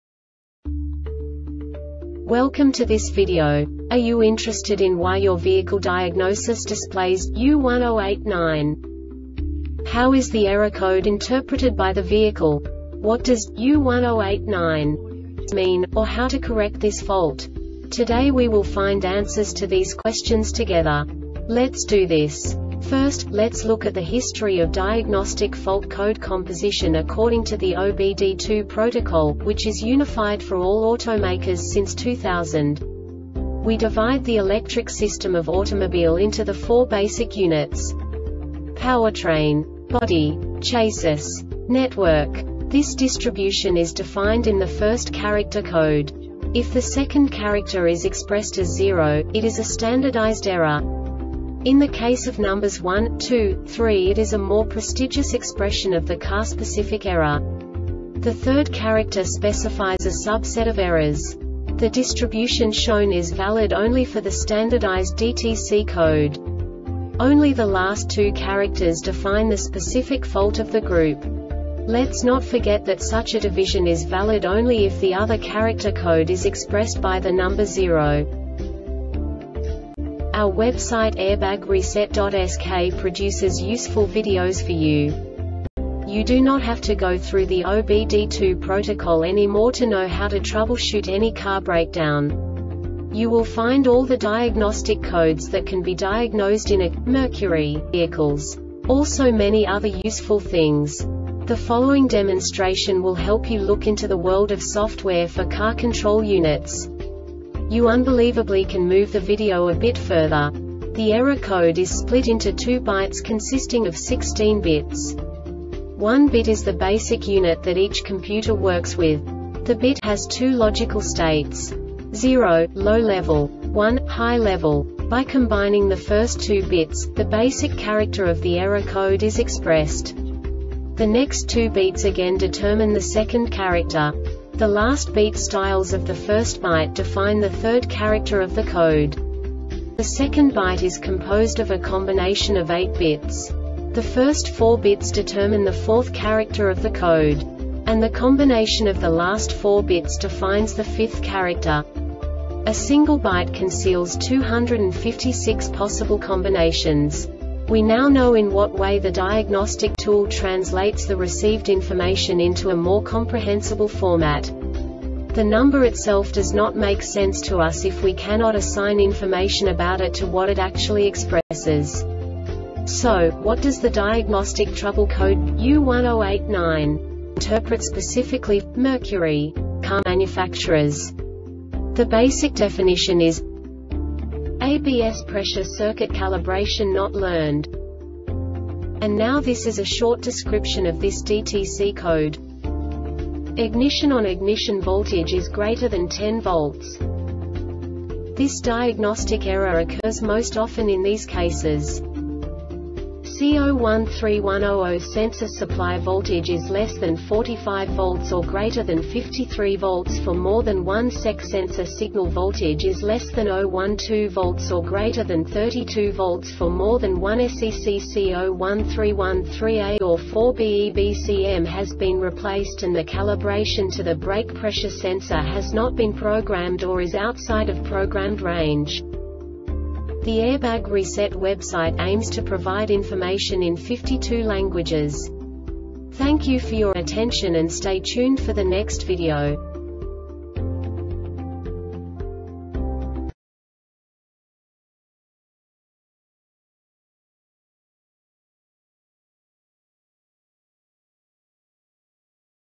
DTC Mercury U1089 Short Explanation
Contents:
0:21 Basic DTC analysis according to OBD2 protocol standard.
1:48 Insight into programming
2:58 A diagnostically understandable description of the Diagnostic Trouble Code
3:05 Basic error definition

Make:
Mercury
Code:
U1089
Definition:
ABS Pressure Circuit Calibration Not Learned
Description:
Ignition ON.Ignition voltage is greater than 10 volts.
Cause:
C0131 00 Sensor supply voltage is less than 4.5 volts or greater than 5.3 volts for more than 1 sec.Sensor signal voltage is less than 0.12 volts or greater than 3.2 volts for more than 1 sec.C0131 3A or 4BEBCM has been replaced and the calibration to the brake pressure sensor has not been programmed or is outside of programed range.